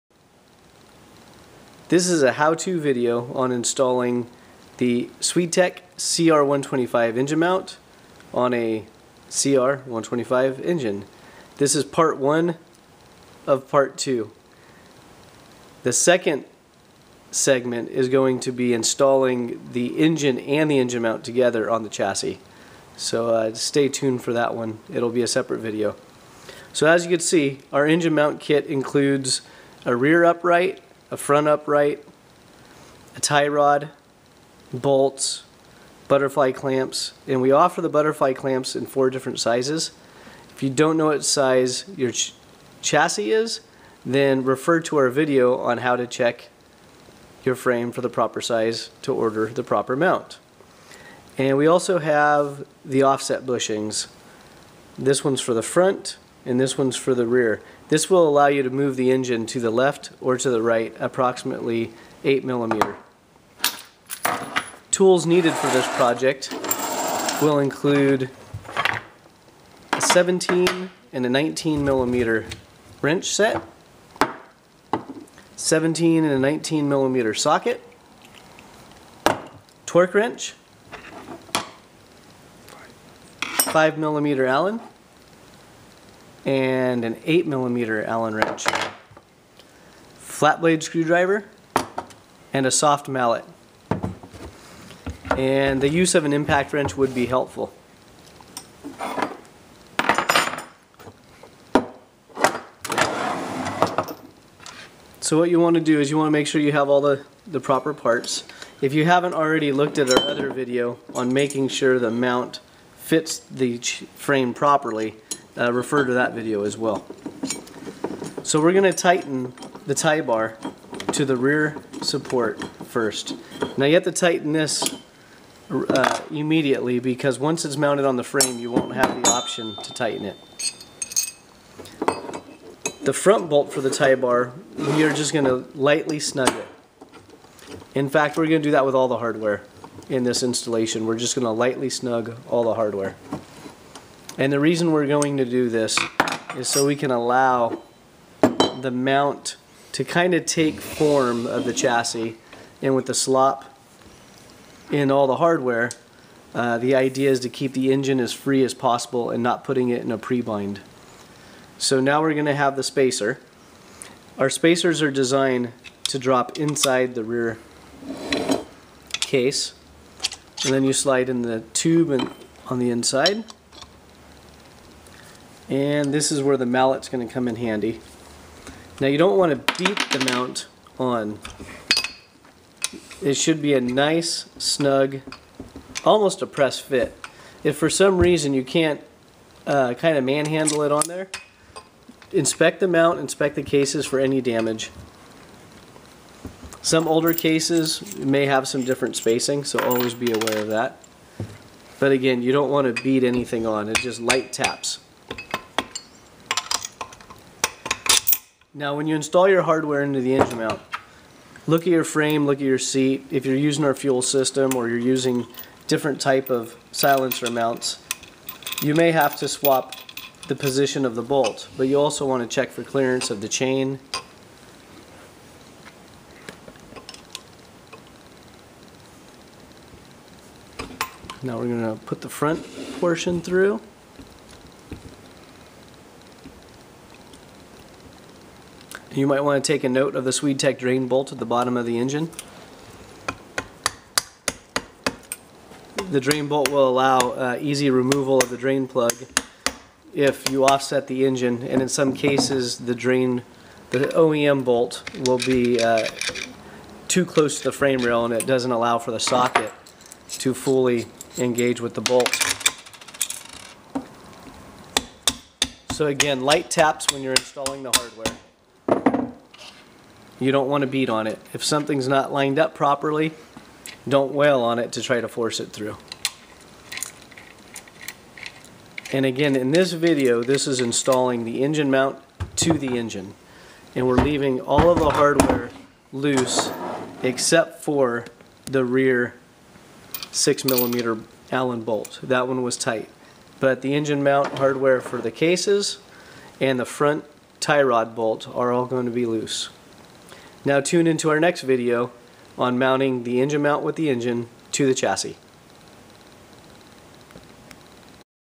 How to Install the SwedeTech CR125 Engine Mount Part 1
How To video instructs you on installing the SwedeTech CR125 engine mount to the engine.  This is a two part video segment - Part 2 is

You can purchase the engine mount directly

Suggested Torque Specs -

10mm butterfly bolts - 45-55 ft /lbs

10mm front bolt - 45 - 55 ft lbs

1/2 Rear Engine mount bolt - 80-90 ft lbs

6mm Allen Bolts - 8 - 10 ft / lbs